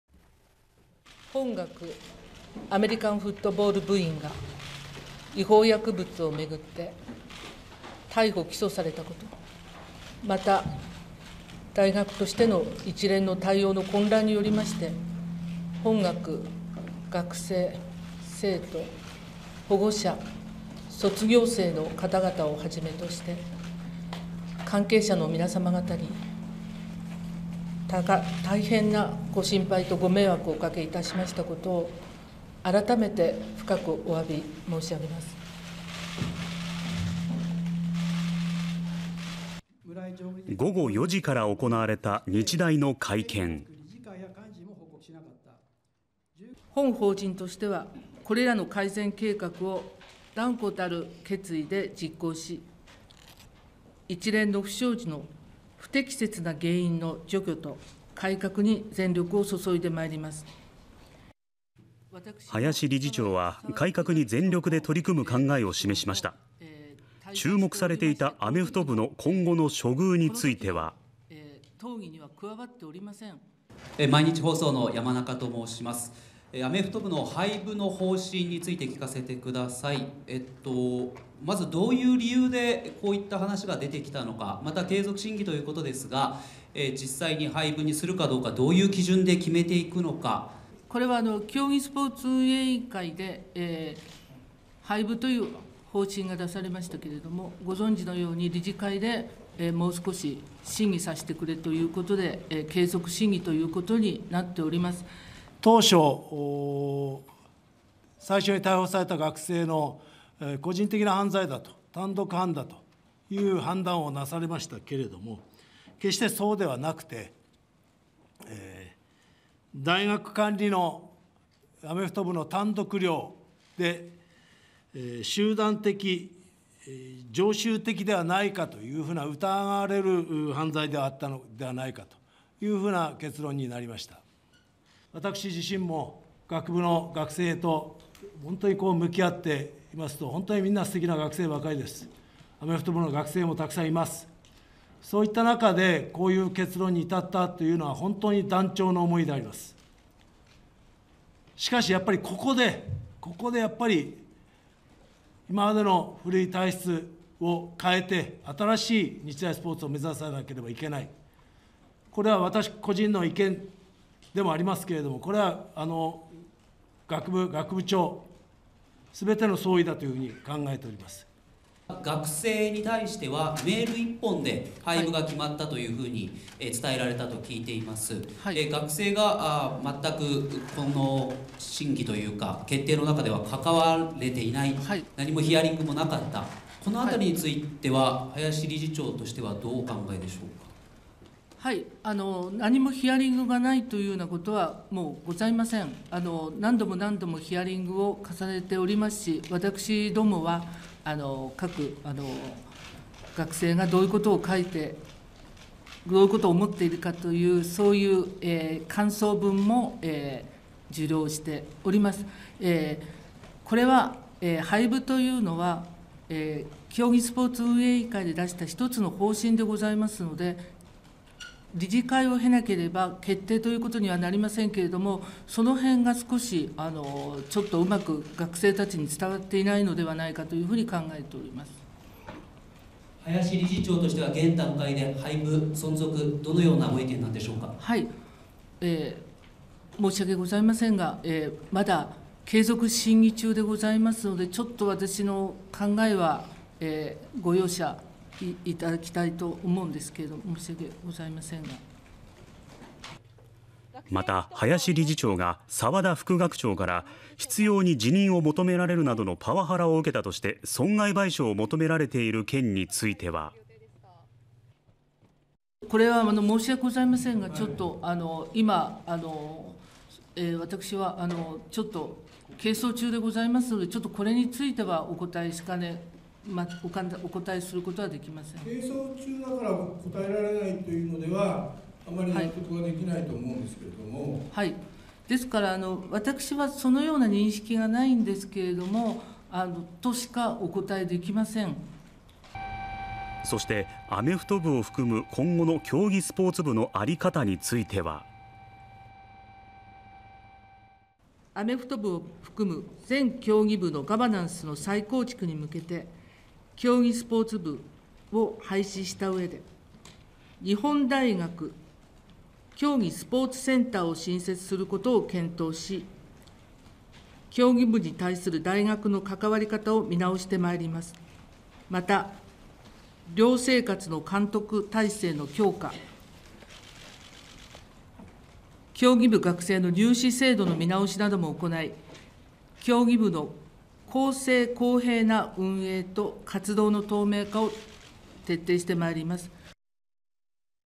林理事長は廃部・存続どんな意見？「継続審議中なので私の考えはご容赦いただきたい」（2023年12月4日）
日本大学アメリカンフットボール部の薬物問題をめぐり、日大の林真理子理事長は１２月４日午後４時ごろからの記者会見で、次のように述べました。

　「本学アメリカンフットボール部員が違法薬物をめぐって逮捕・起訴されたこと、また大学としての一連の対応の混乱により本学学生生徒・保護者・卒業生などをはじめとする関係者の皆さま方に、大変なご心配とご迷惑をおかけしたことを改めて深くお詫び申し上げます。本法人としてはこれらの改善計画を断固たる決意で実行し、一連の不祥事の不適切な原因の除去と改革に全力をそそいでまいります」

　林理事長は改革に全力で取り組む考えを示しました。注目されていたアメフト部の今後の処遇については…

　「（Ｑ廃部の方針について理由と基準は？）競技スポーツ委員会で廃部という方針が出されましたけれども、理事会でもう少し審議させてくれということで、継続審議になっております」

　再発防止策検討委員会の益子俊志委員長は次のように述べました。

　「当初、最初に逮捕された学生の個人的な犯罪、単独犯だと判断をされましたけれども、決してそうではなくて、大学管理のアメフト部の単独寮で集団的・常習的ではないかと疑われる犯罪ではないかという結論になりました。私自身も学生と向き合う中で、こういう結論に至ったというのは本当に断腸の思いであります。しかしここで今までの古い体質を変えて新しい日大スポーツを目指さないといけない。これは私個人の意見でもありますが、学部・学部長全ての総意だと考えております」

　林理事長は、記者側から「学生への廃部連絡はメールで伝えられたと聞いている。学生がこの審議の決定に関わることができていない、ヒアリングもなかったとされる点についてどうお考えか？」と問われると…

　「何もヒアリングないということはございません。何度も何度もヒアリングを重ねていますし、私どもは各学生がどういうことを書いてどういうことを思っているかという感想文も受領しております。廃部というのは競技スポーツ運営委員会が出した１つの方針でございますので、理事会を経なければ決定ということになりません。そのへんが少しうまく学生に伝わっていないのではないかと考えております。（Ｑ理事長としては廃部・存続、どのようなご意見？）申し訳ありませんが、まだ継続審議中ですので、ちょっと私の考えはご容赦いただきたいと思います」

　一方、執ように辞任を求められるなどのパワハラを受けたとして、沢田副学長から損害賠償を求められている件について、林理事長は…

　「これは申し訳ございませんが、今私は係争中でございますので、これについてはお答えすることはできません。（Ｑ答えられないというのでは納得ができないが？）私はそのような認識がないとしかお答えできません」

　そして、アメフト部を含む、今後の競技スポーツ部のあり方については、次のように述べました。

　「アメフト部を含む全競技部のガバナンスの再構築に向けて、競技スポーツ部を廃止したうえで、日本大学競技スポーツセンターを新設することを検討し、競技部に対する大学の関わり方を見直してまいります。また寮生活の監督体制の強化、競技部学生の入試制度の見直しなども行い、競技部の公正・公平な運営と活動の透明化を徹底してまいります」